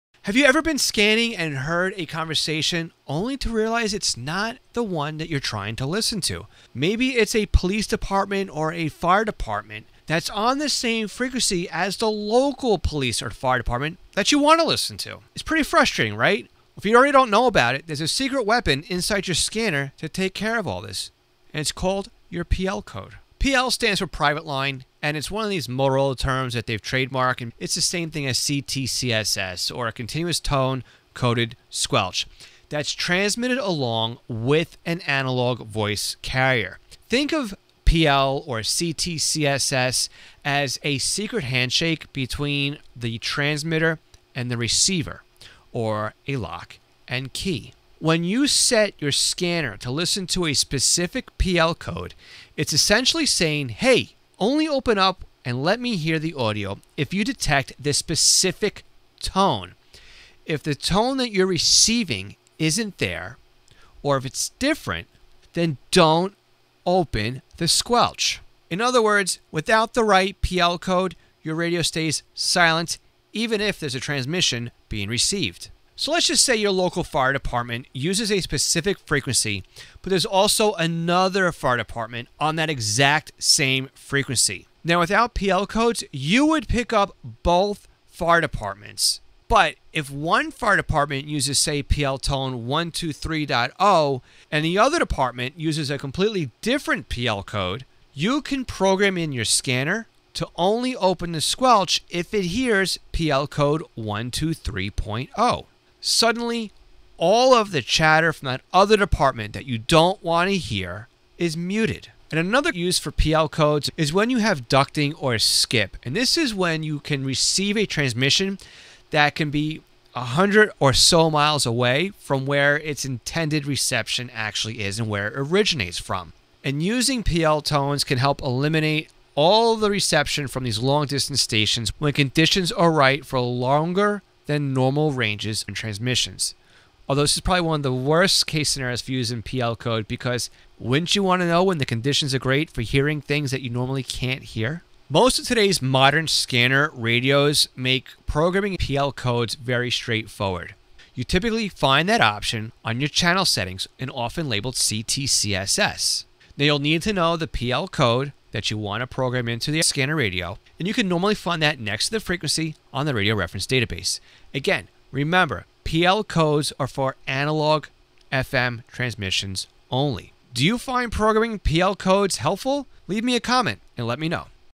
PL Codes Explained
Learn how PL codes (Private Line codes) help you filter out unwanted radio chatter, avoid interference, and only hear the transmissions you want.

Found this helpful? Drop a comment and follow for more tips!